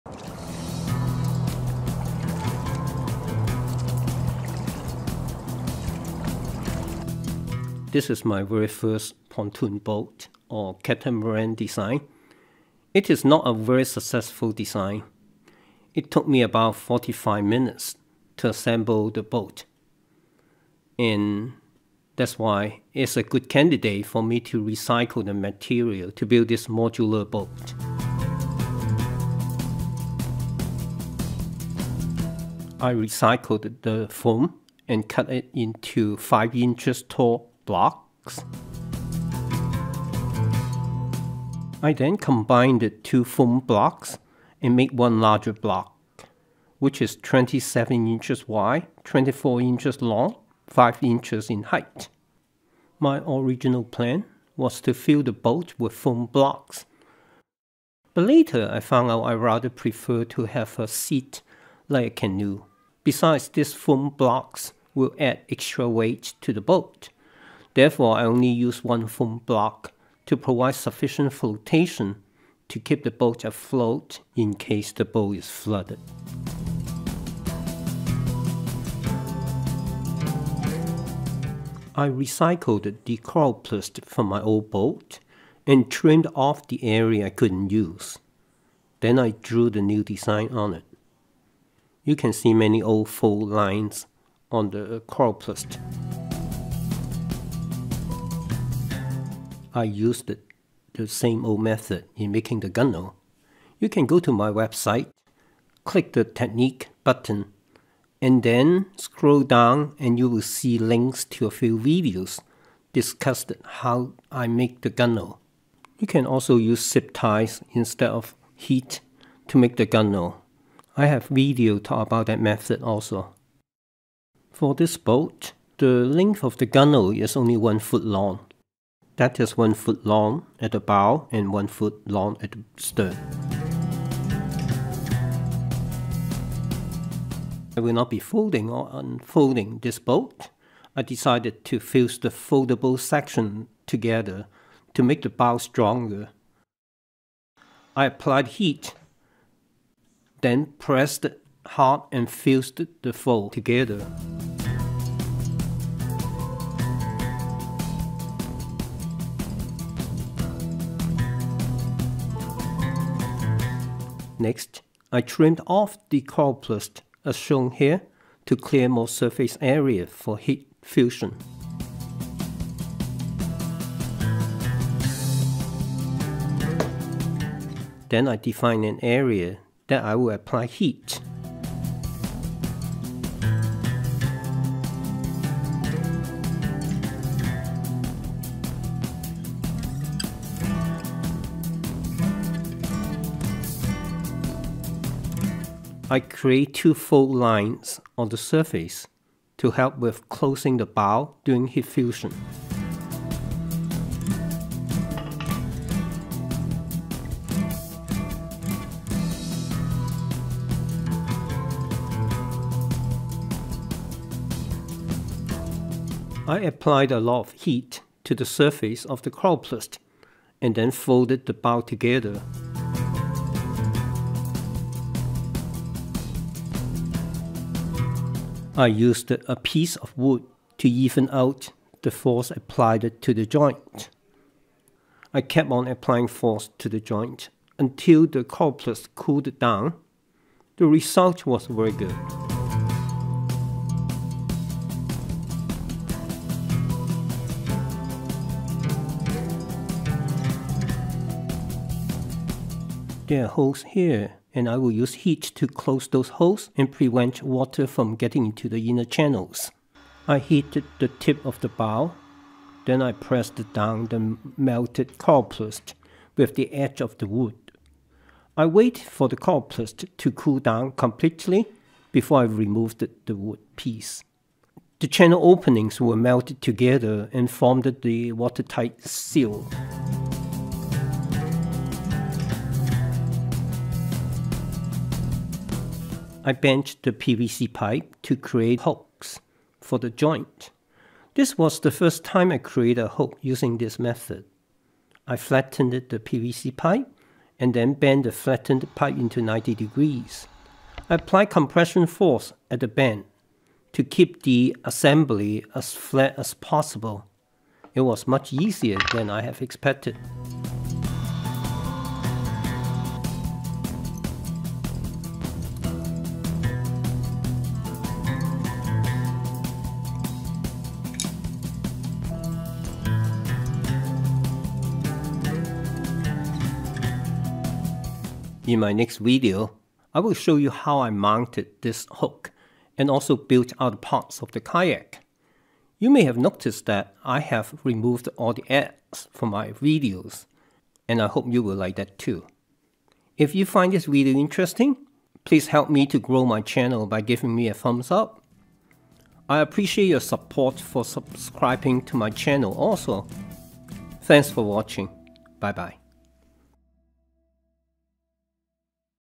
Pt. 2 Build A 10 ft. Coroplast Modular Kayak Gen 20
The is a coroplast modular kayak. This material - coroplast, allows me to build a kayak weighs less than 20 lbs. The semi-rigid hull performs better than my folding kayaks. I can also shape the hull and make it more hydrodynamic. This boat is more reliable than my folding kayaks.
I am going to provide more details in building this kayak in this video series. My first build is not prefect and I will discuss how to build a better modular boat in this series.
Part 1: Advantages of the design of this kayak
Part 2: Build the bow & stern section of the kayak